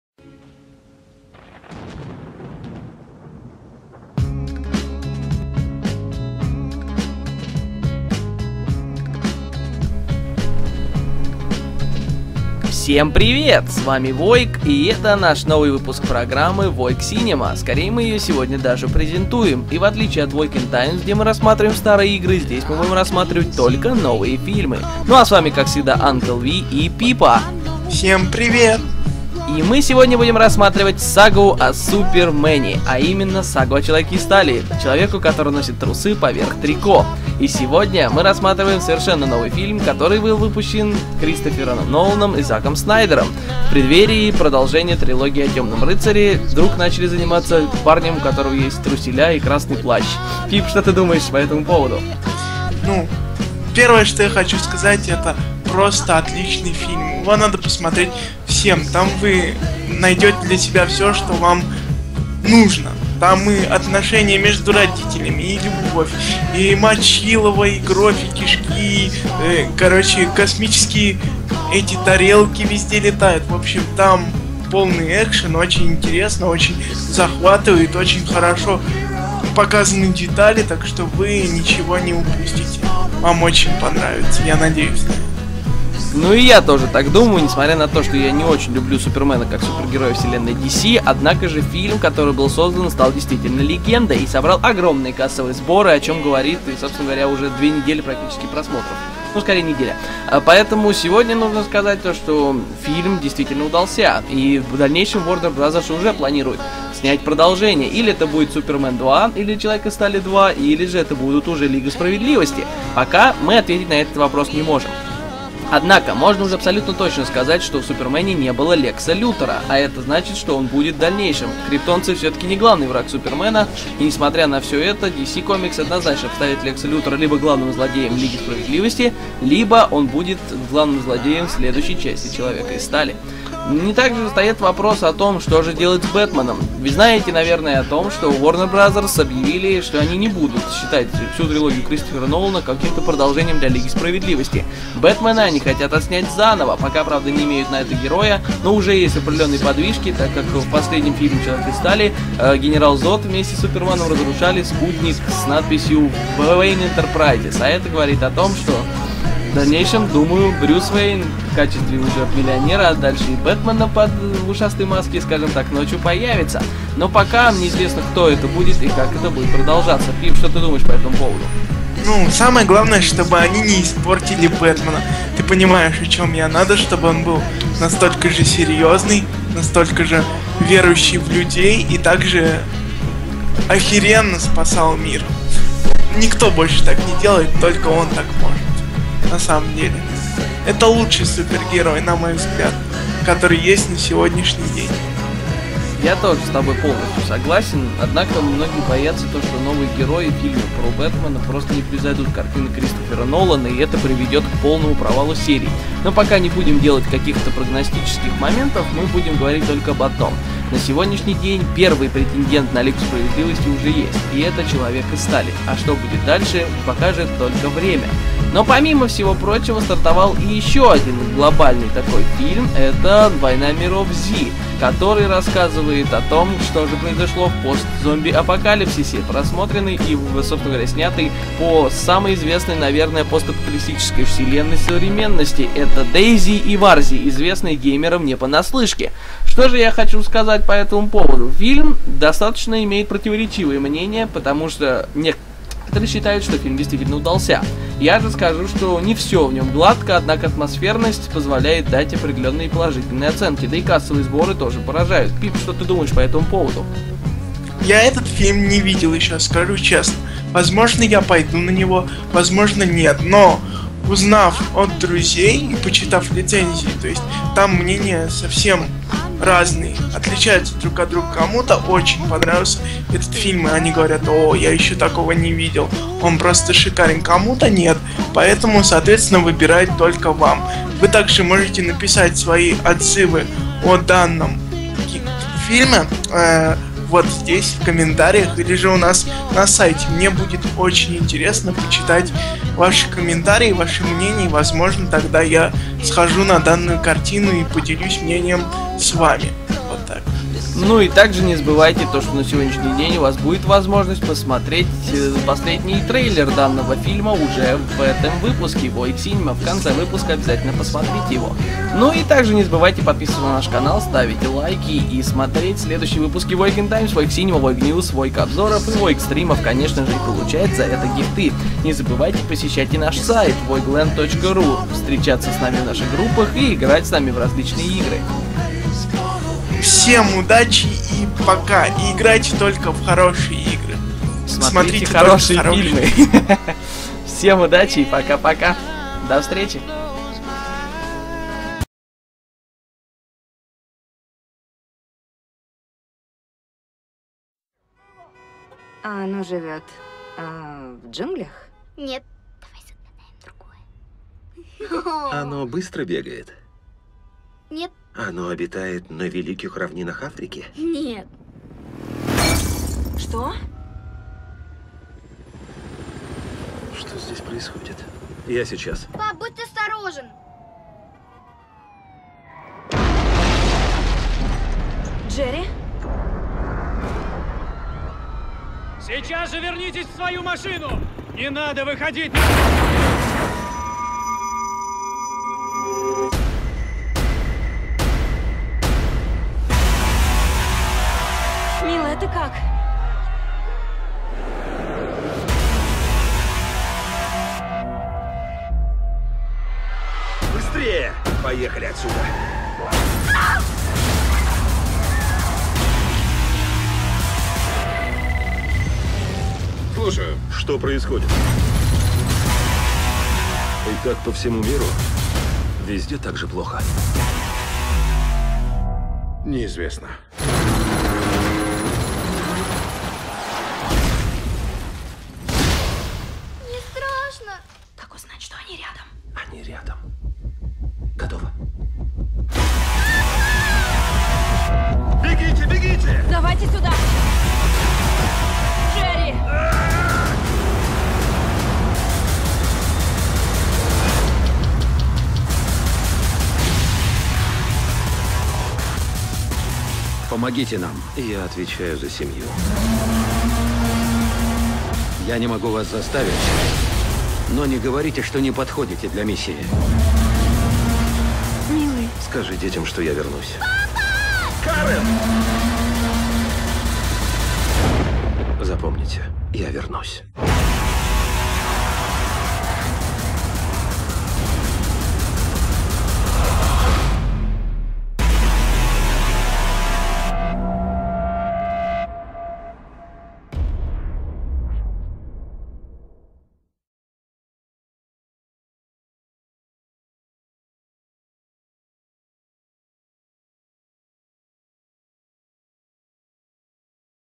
WOIG Cinema Begins
Презентация проекта WOIG Cinema и новые фильмы проката а также мнение о них от Uncle V  и Pipa
Не забываем посмотреть предыдущие обзоры!